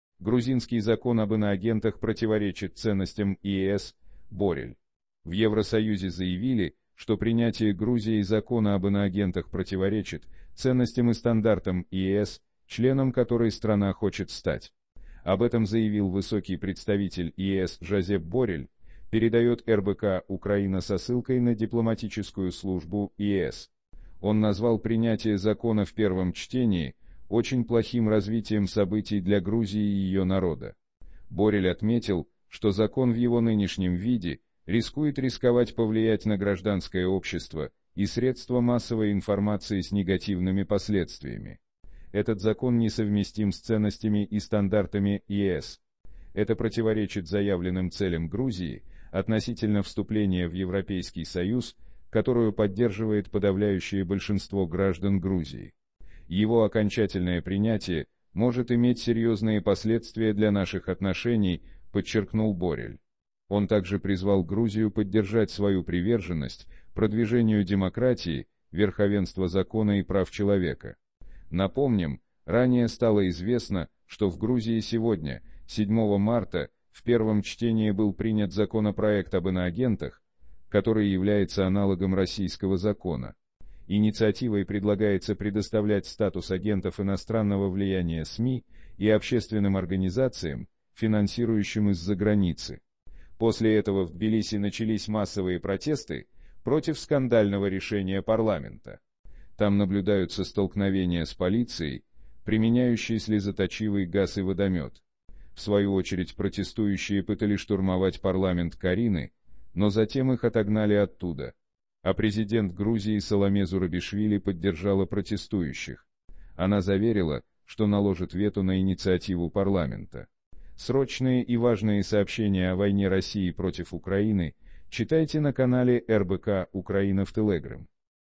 Грузинский закон об "иноагентах" противоречит ценностям ЕС, - Боррель.
Грузия Евросоюз В Евросоюзе заявили, что принятие Грузией закона об "иноагентах" противоречит ценностям и стандартам ЕС, членом которой страна хочет стать. Об этом заявил высокий представитель ЕС Жозеп Боррель, передает РБК-Украина со ссылкой на Дипломатическую службу ЕС. Он назвал принятие закона в первом чтении "очень плохим развитием событий для Грузии и ее народа". Боррель отметил, что закон в его нынешнем виде рискует рисковать повлиять на гражданское общество и средства массовой информации с негативными пос